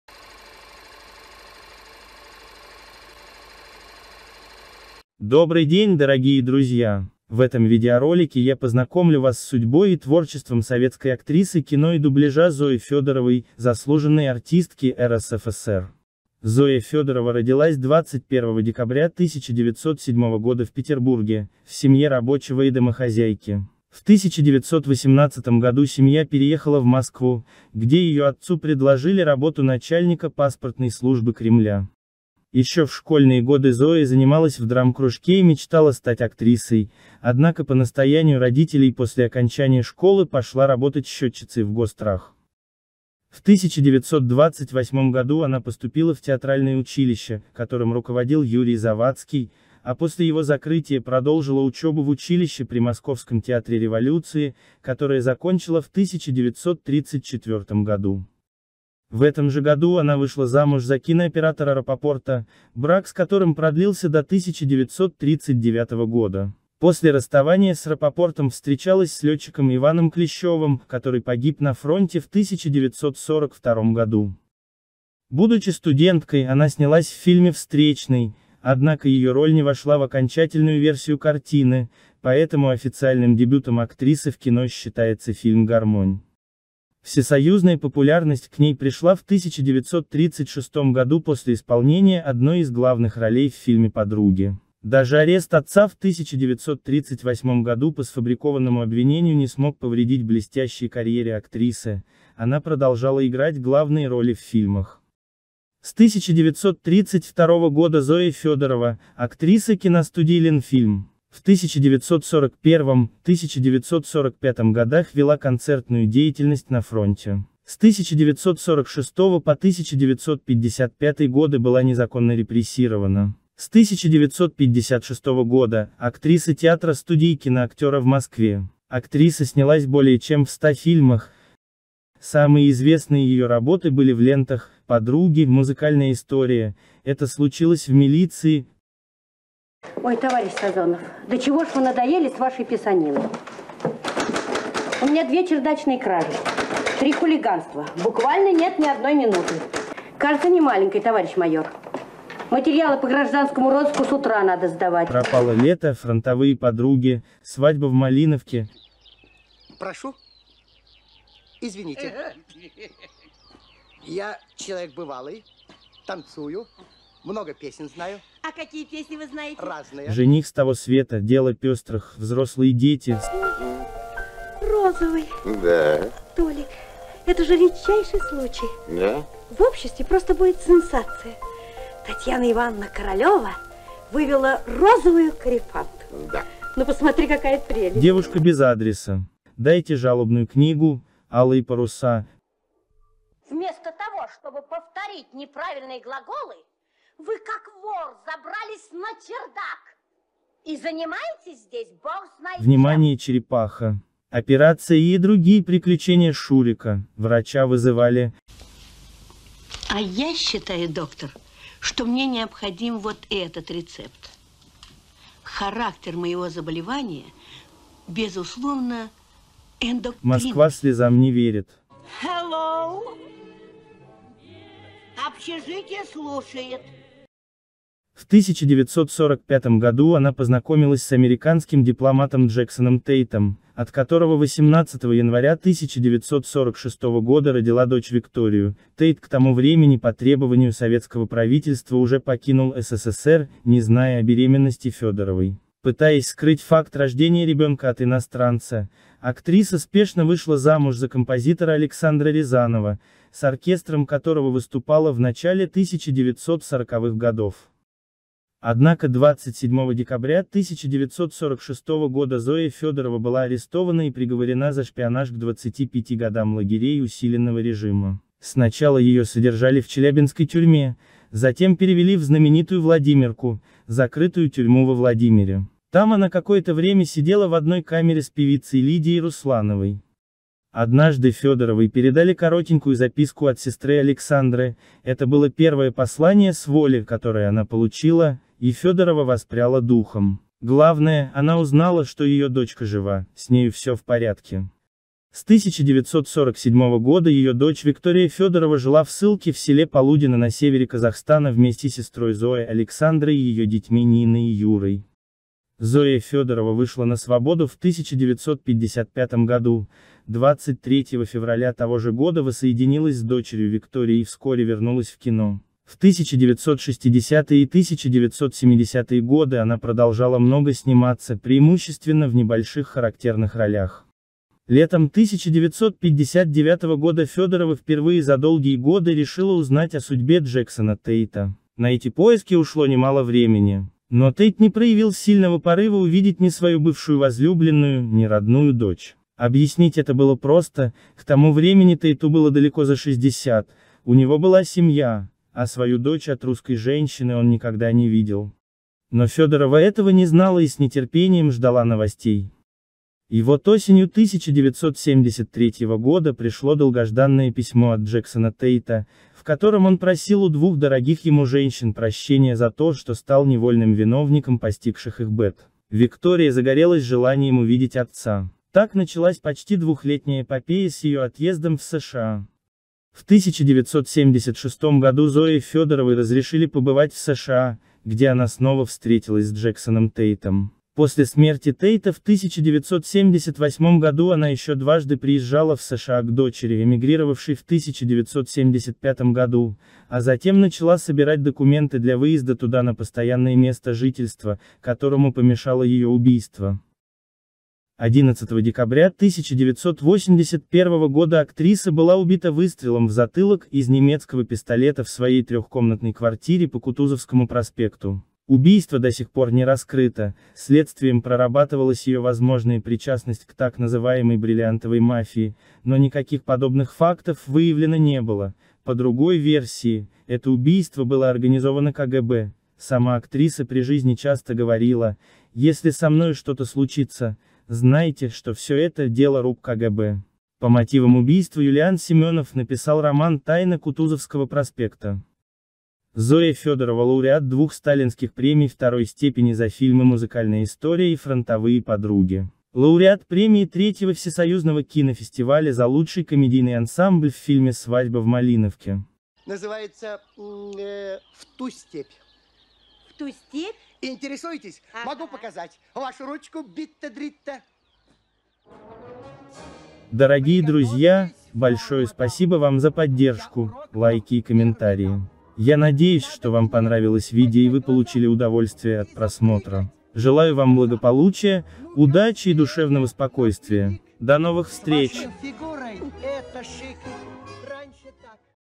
Зоя Фёдорова. ЖиЗнЛ. Судьба и творчество...
Добрый день дорогие друзья! В этом видеоролике я познакомлю Вас с судьбой и творчеством советской актрисы кино и дубляжа Зои Фёдоровой. Заслуженной артистки РСФСР.
🙏 Добавить видео на свой канал, страницу или любой сайт, переслать друзьям или поделиться можно по ссылке